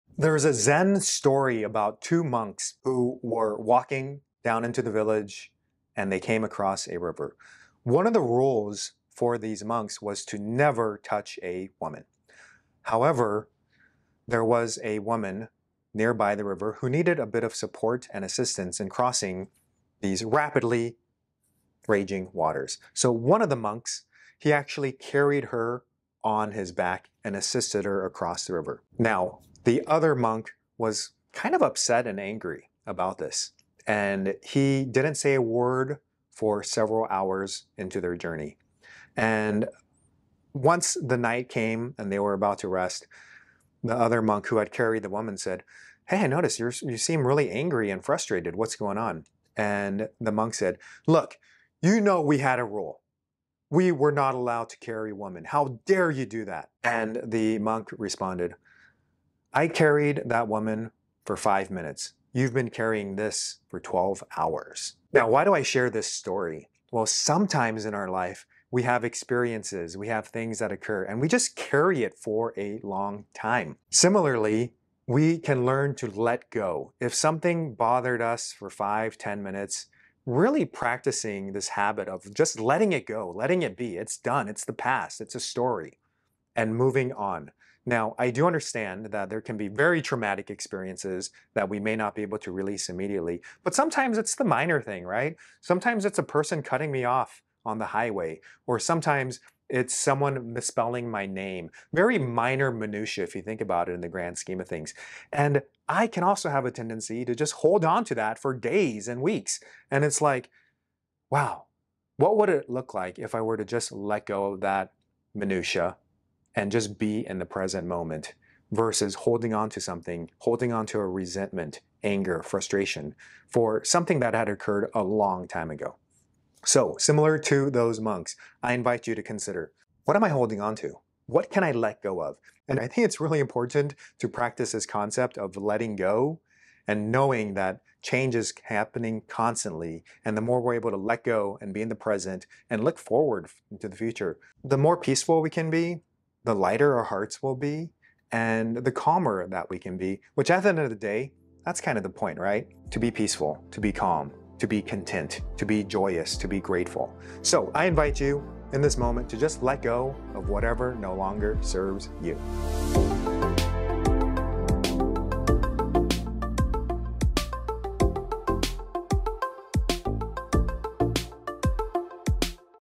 What Have You Been Carrying For Too Long: The Story of Two Monks
Explore the Zen of Letting Go in this video with Dr. Ko. Uncover the transformative wisdom in the story of two monks and a moment of compassion that challenges our self-imposed rules. It is not just a lesson from the past. It's a mirror reflecting our own tendency to hold onto things far longer too long.

🔍Why Watch? Whether it’s a major life event or a minor annoyance, we all carry something we could let go of. Join Dr. Ko as he guides us through this story’s enduring wisdom, encouraging us to reflect on what we’re carrying and how to lighten our load.